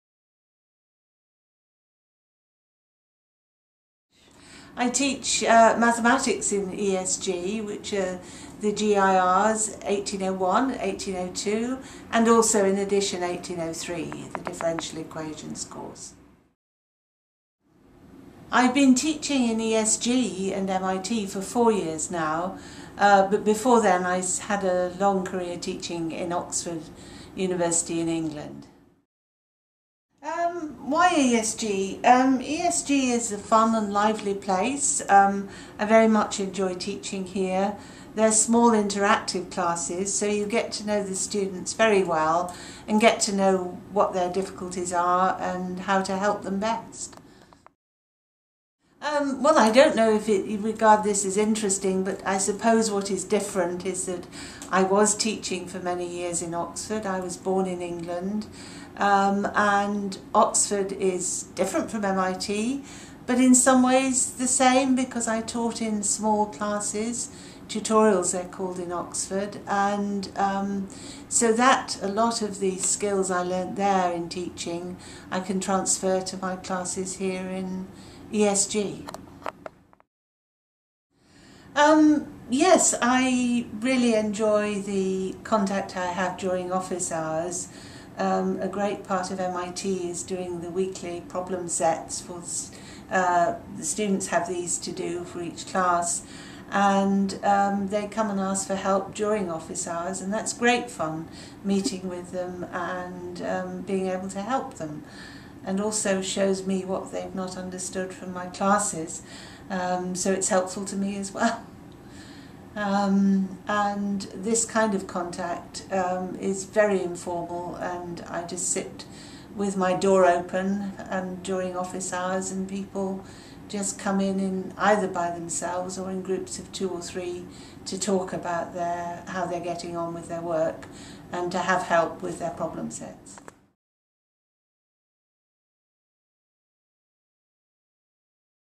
Staff interview with Gabrielle Stoy
Paula interviews ESG math staff member Gabrielle Stoy.